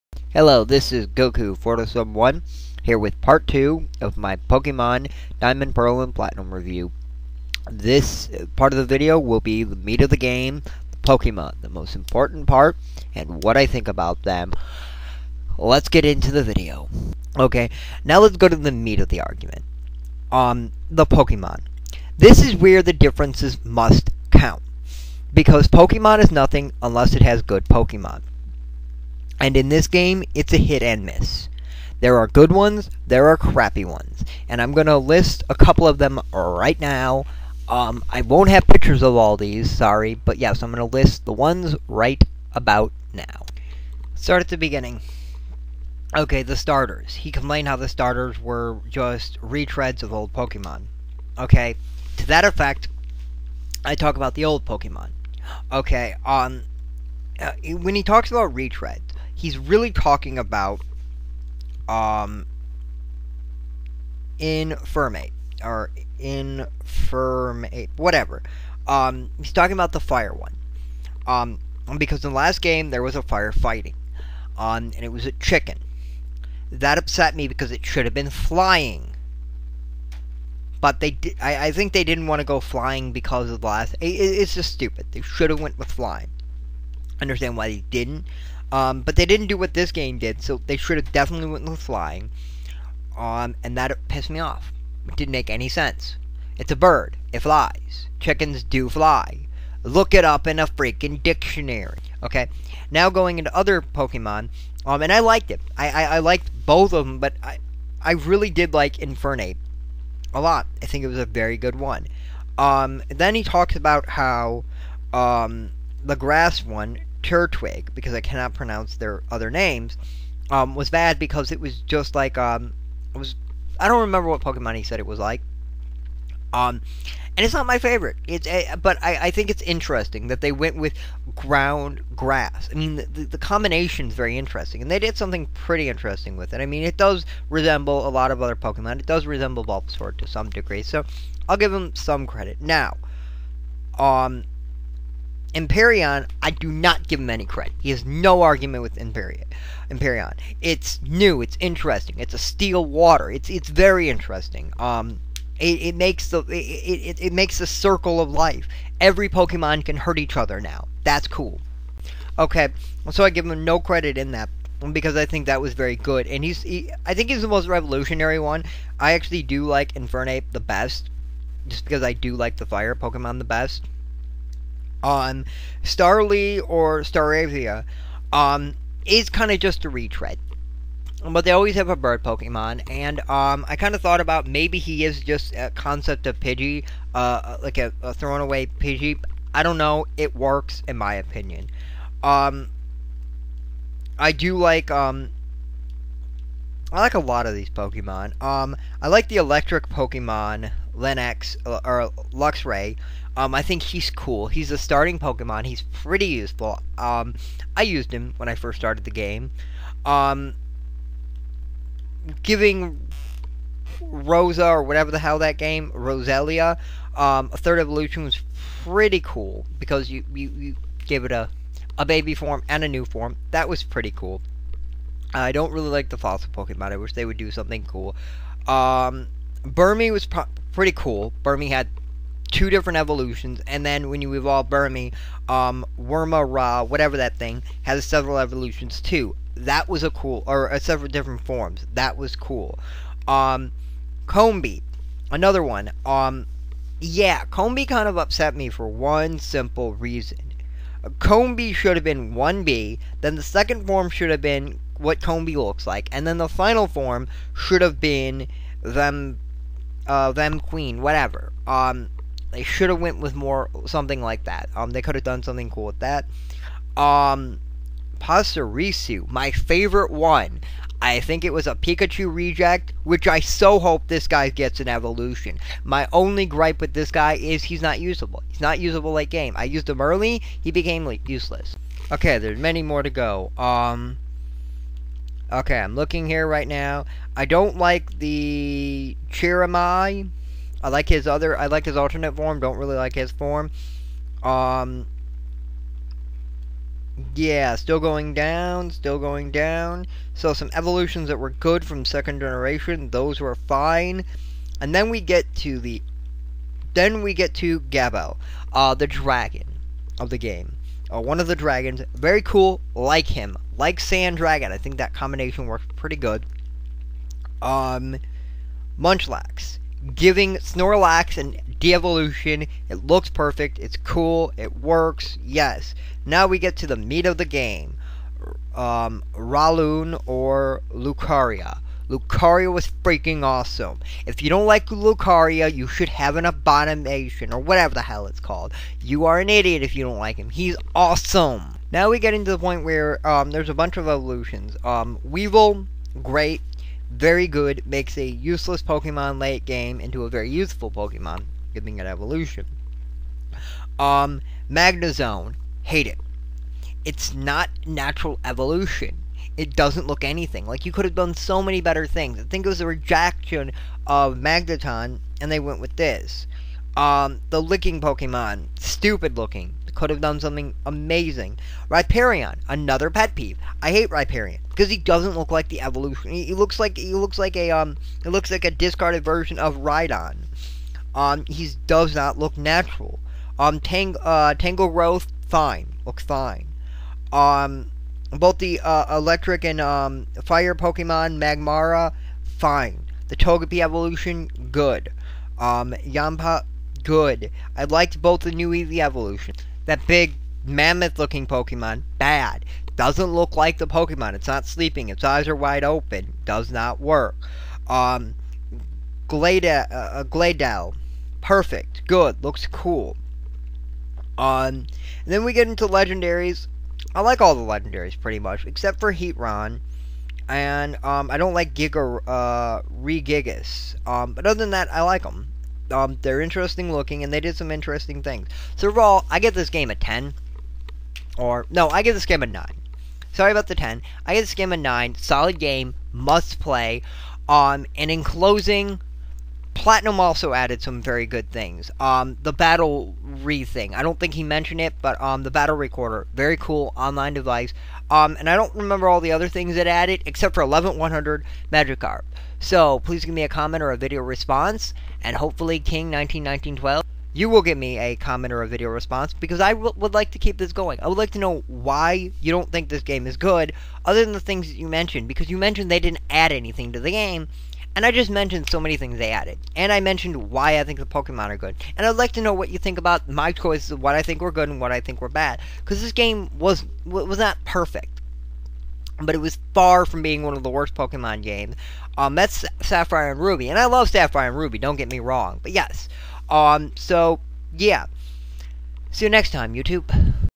Pokemon Diamond Pearl and Platinum Review Part2
A video reply to king191912 Pokemon Pearl and my thoughts